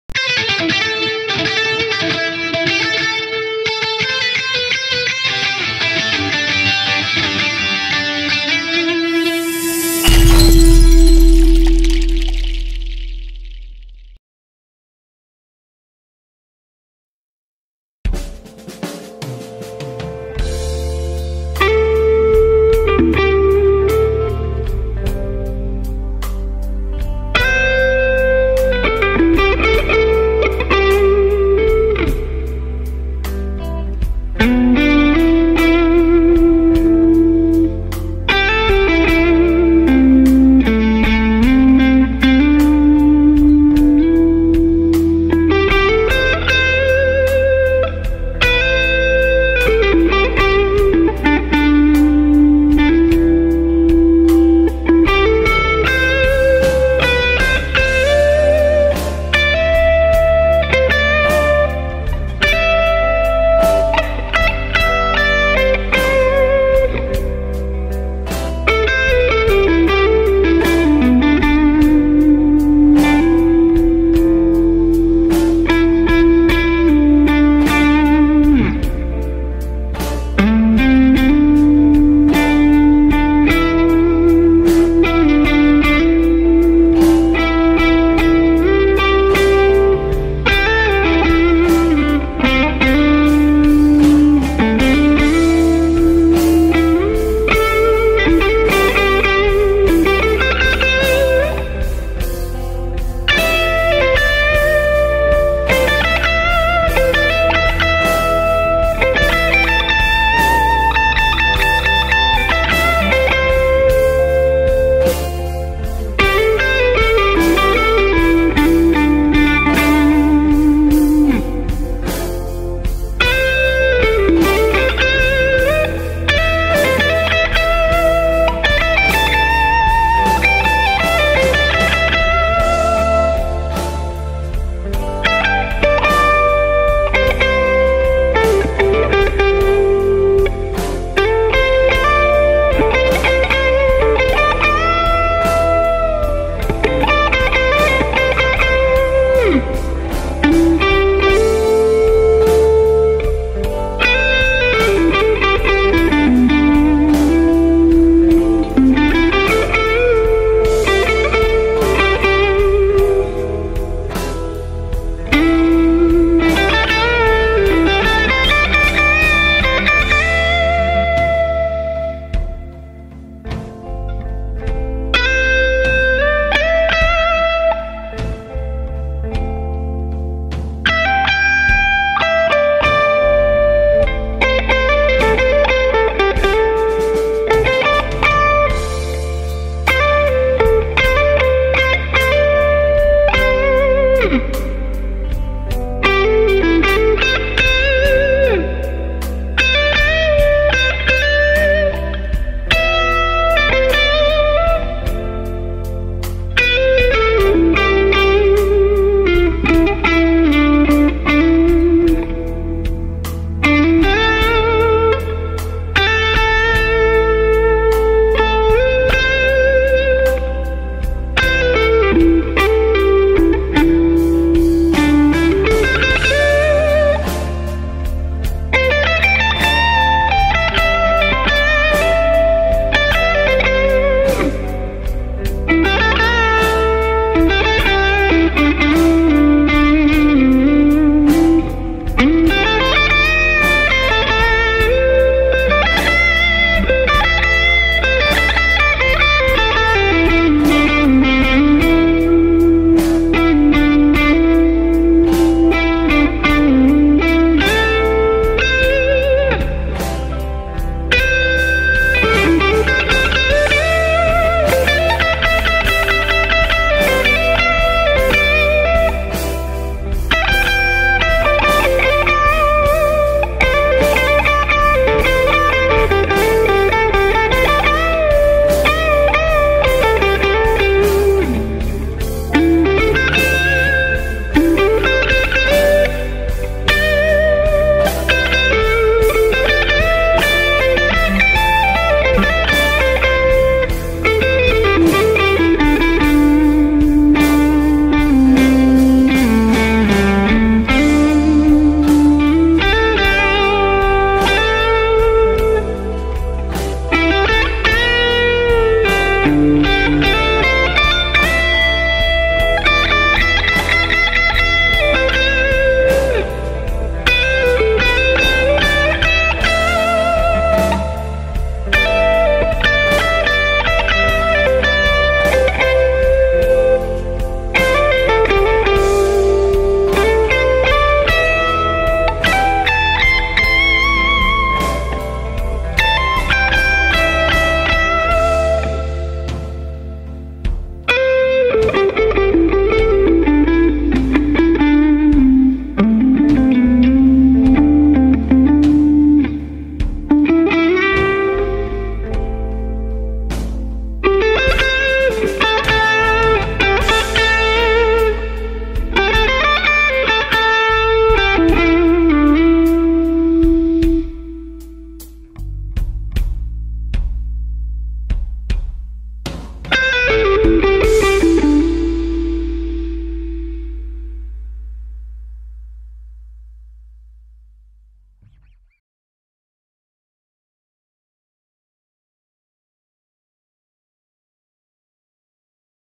Blues Ballad in A Minor
Halloooww guys! Here's my blues guitar jam in A Minor! I hope you'all enjoy this! Stay Safe & God bless!